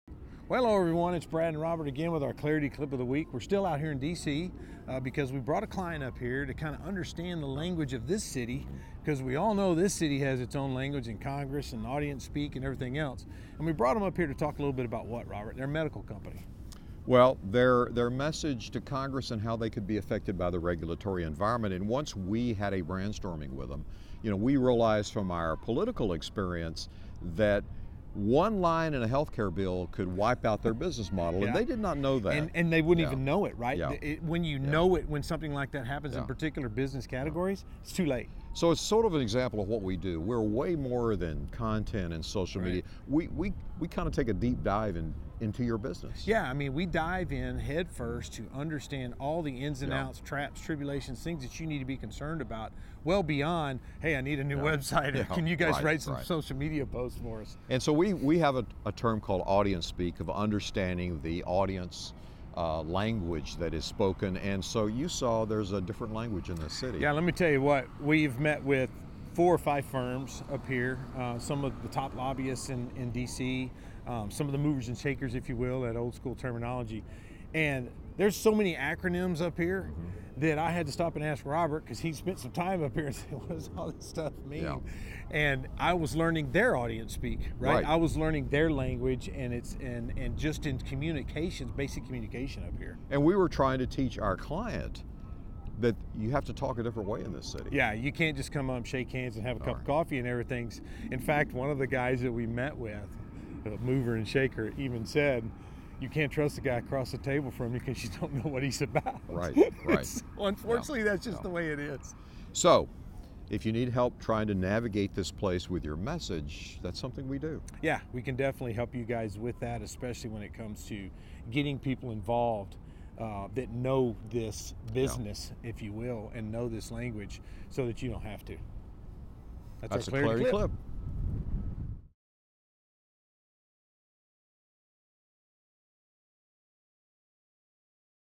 How Clarity Digital Marketing Helps Clients Learn The Language Of Politics
We consider ourselves a SWAT team when it comes to identifying threats to the prosperity of our client's business.
Here's how we help them talk the language of Congress.